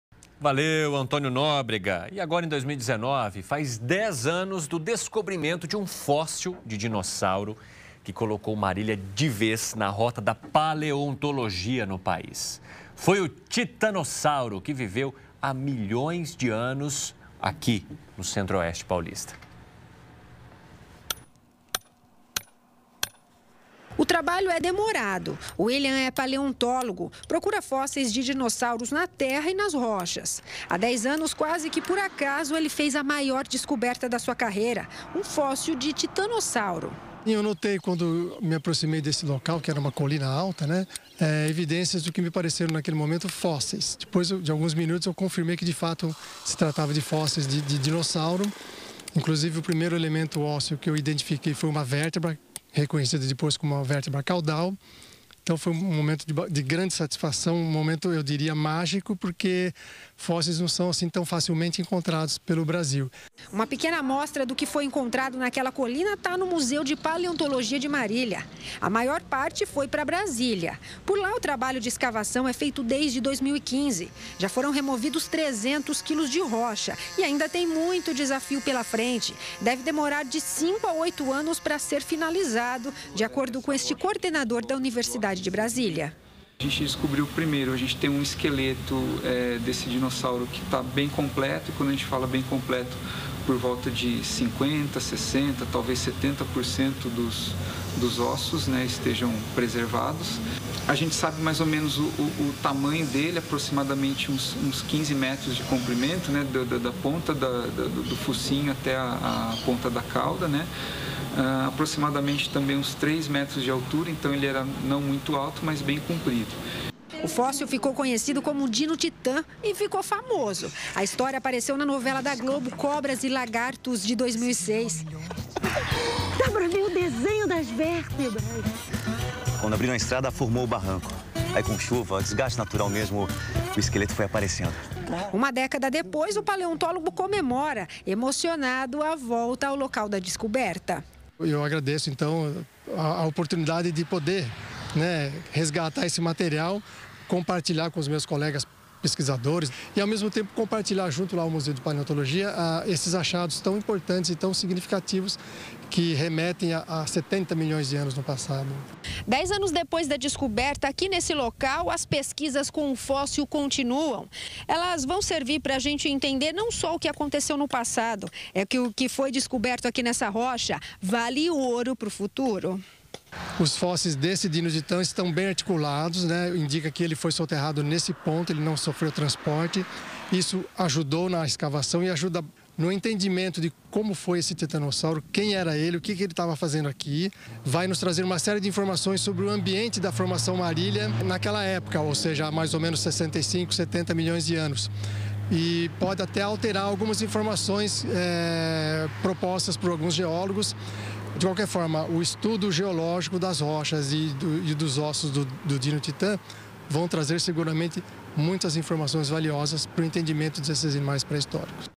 DINO TITÃ
DINO TITÃ, 2022

Espécie representada foi encontrada em Marília (SP) e viveu há 70 milhões de anos. A réplica do Dino Titã, com 3,90 metros de altura por 12 metros de comprimento, pesa quase uma tonelada.